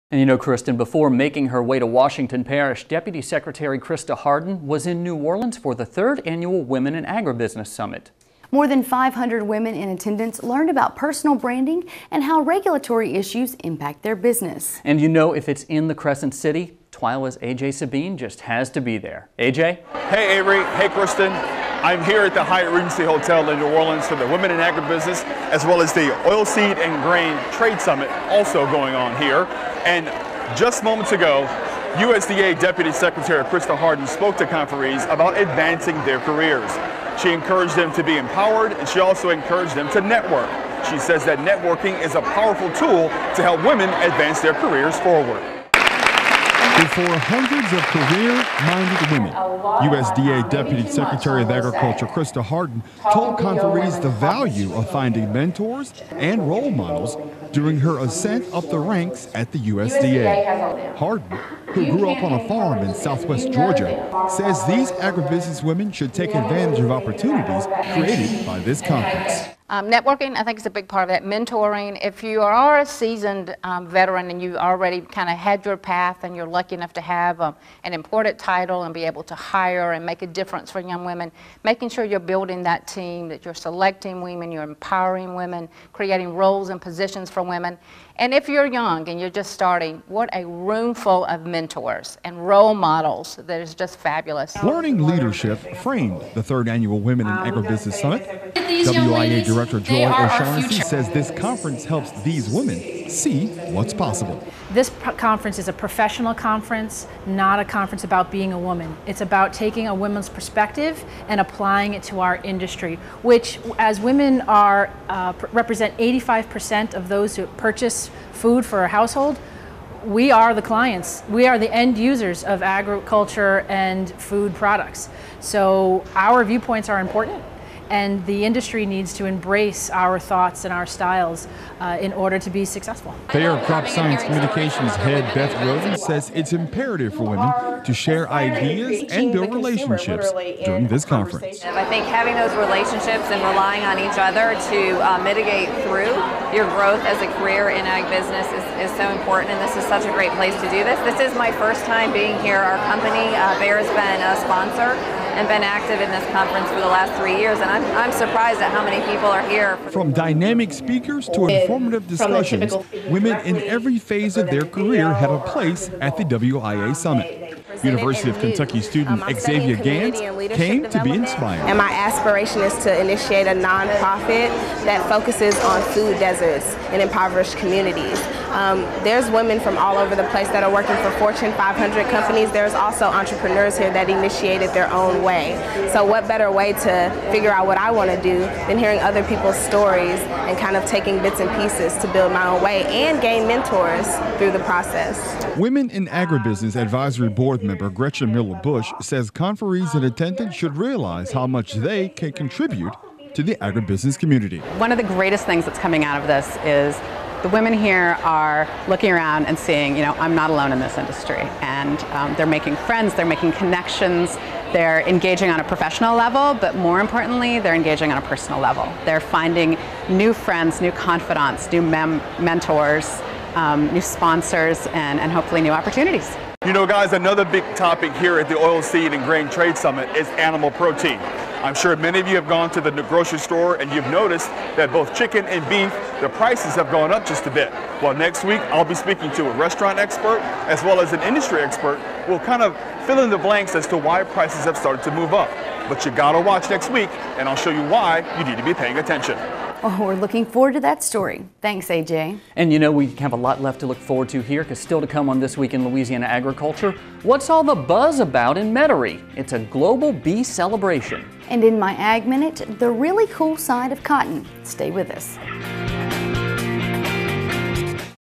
Women Working in Agriculture
Women play a huge role in agriculture. TWILA's A.J. Sabine takes us to the 3rd Annual Women in Agribusiness Summit in New Orleans where women learn about personal branding and how regulatory issues impact their businesses.